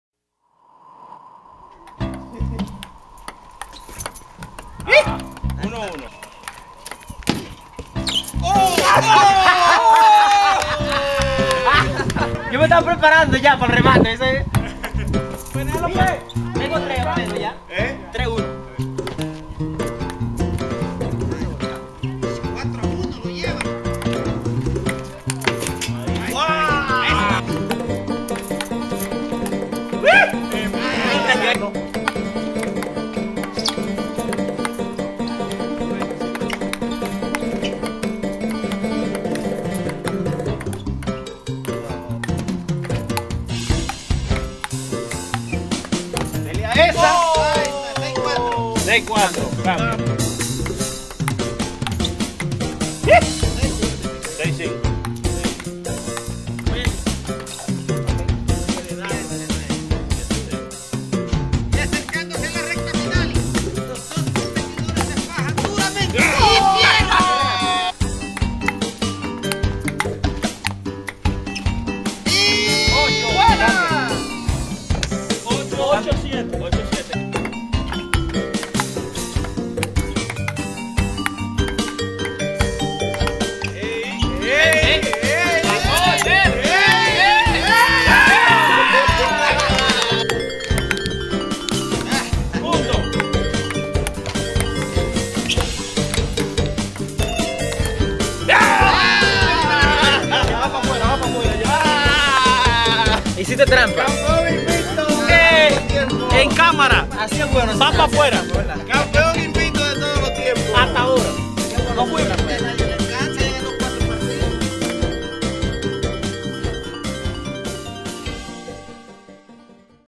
Dauris vs. Pedro en ping pong [Camp Cherokee]
Ping pong en Camp Cherokee, August 2011